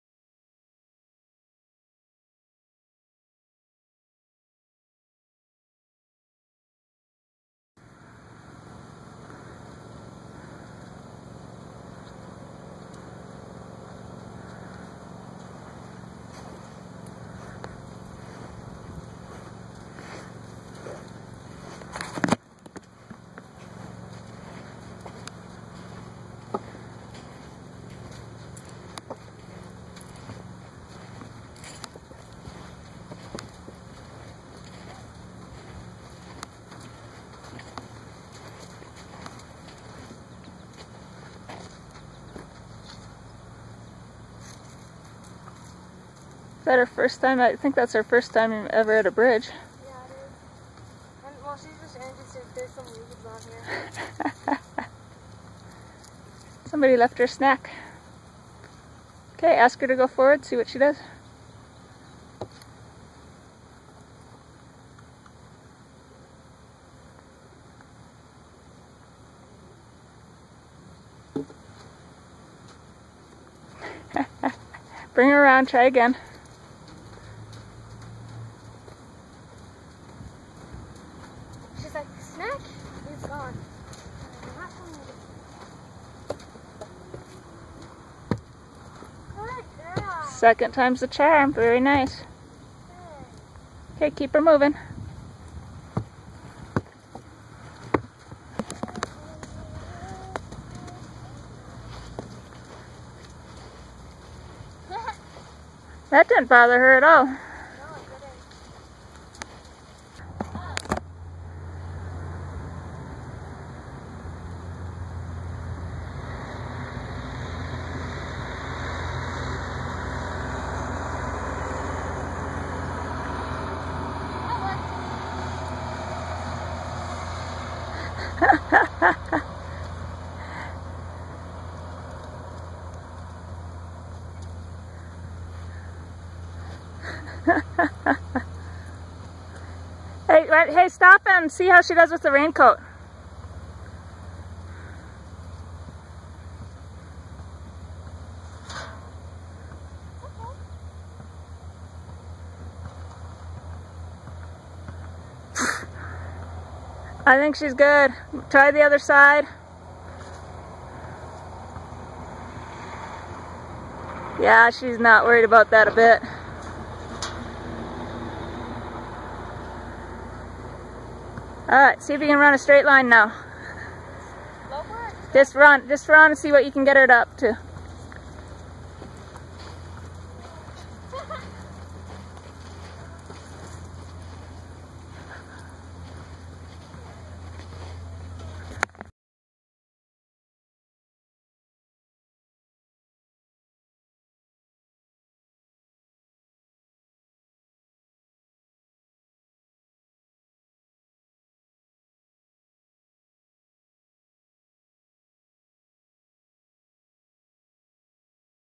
Xoe & the Obstacle Course
We took Xoe with us to the Madison County 4-H Horse Show, just to expose her to new things.  After the trail class finished, we slipped into the arena to see how Xoe would handle the obstacles.  She did great!  You'd never guess it was the first time she had seen these things.